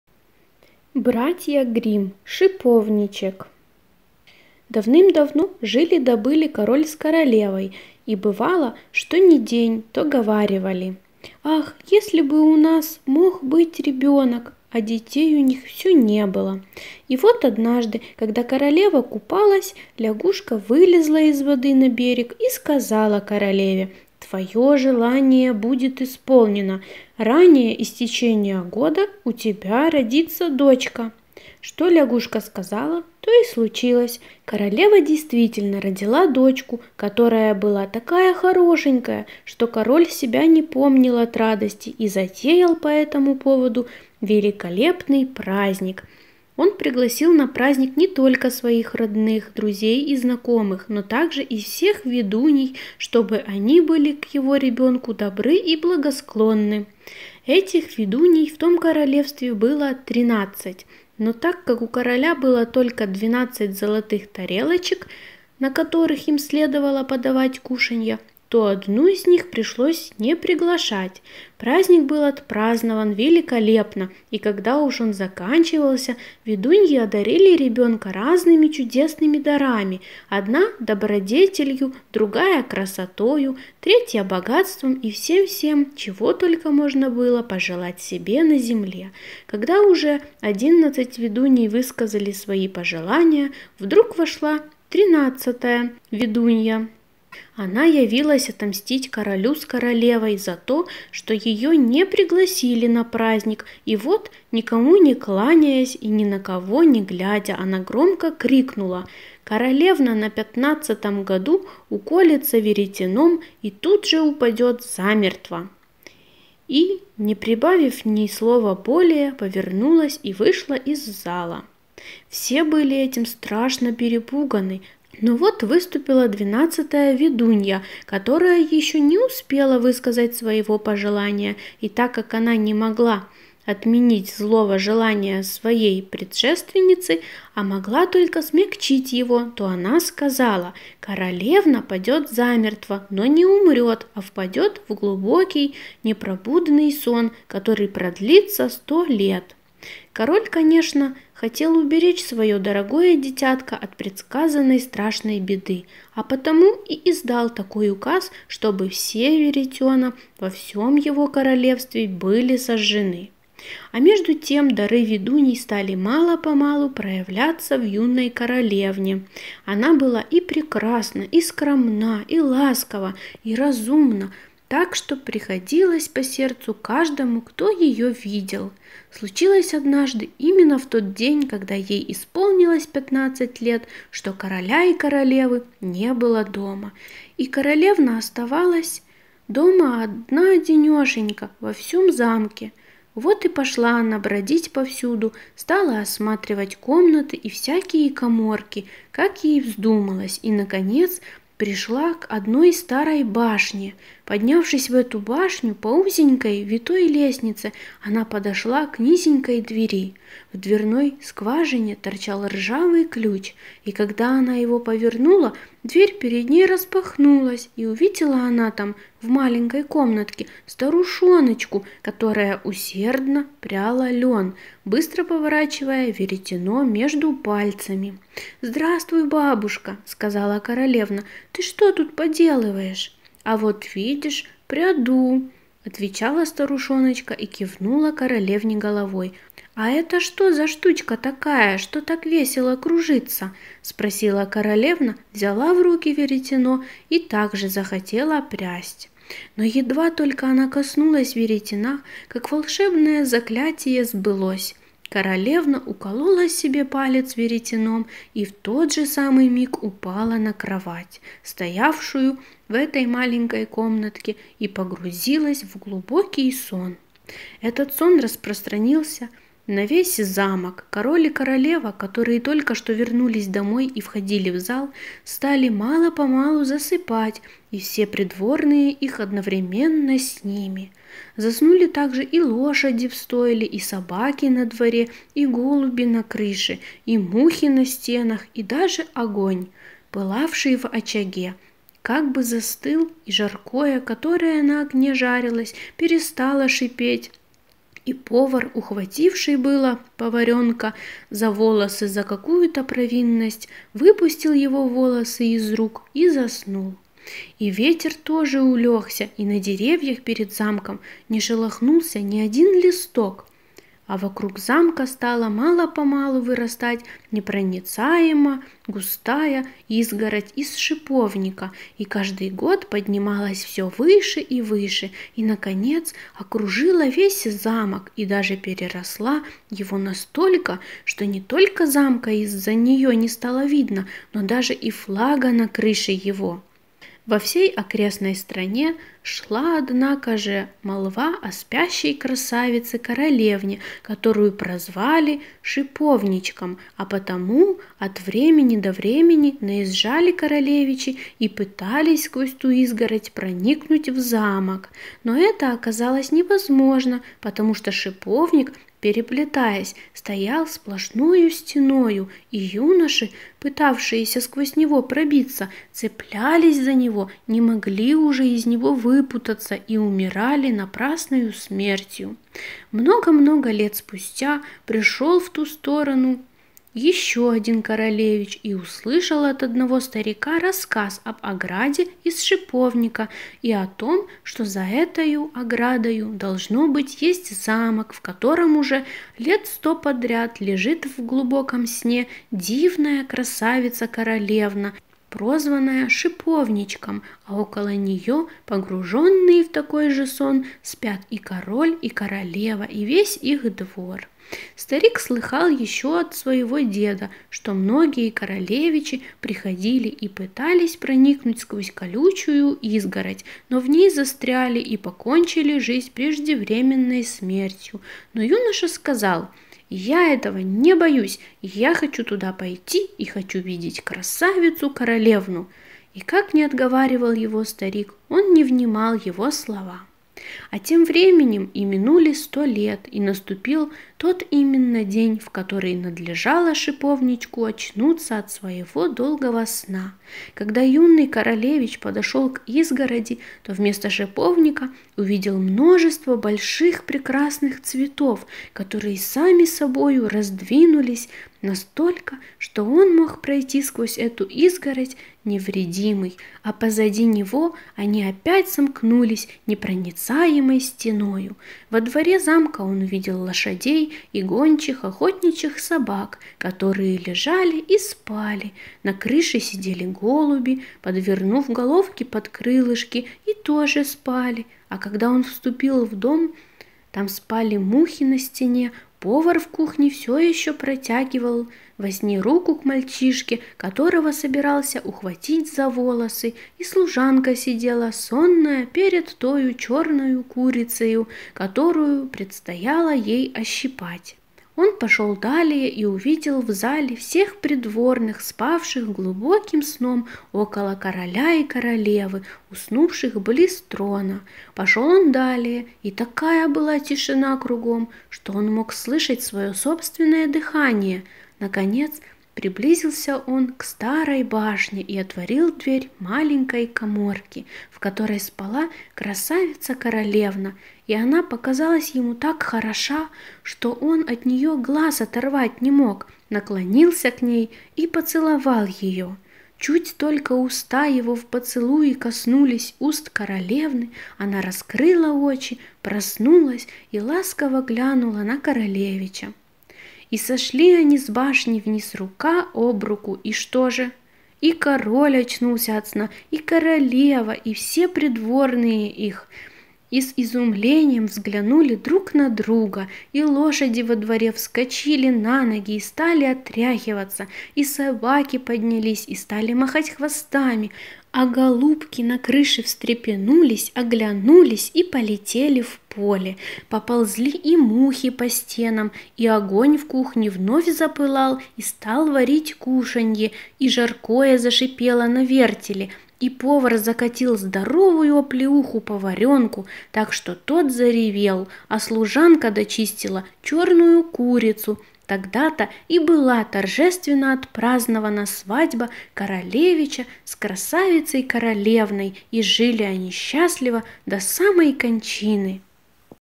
БРАТЬЯ ГРИММ - Шиповничек
Историю, которая схожа со знаменитой «Спящей красавицей», расскажет детям сказка «Шиповничек» братьев Гримм. Как-то раз жили король с королевой, и у них не было детей. Но вот во время купания из воды вылезла лягушка, и пообещала, что у нее будет ребеночек. Через время родилась дочь, и король устроил пир, на который пригласил множество гостей, а также колдуний. Но так как одну колдунью не пригласили, то она решила отомстить, и прокляла дитя. В 15 лет она должна будет уколоться об веретено, а затем испустить дух. Одна из волшебниц смягчила этот приговор: девочка не умрет, а заснет. Так все и случилось. В день своего 15-летия девушка нашла маленькую каморку...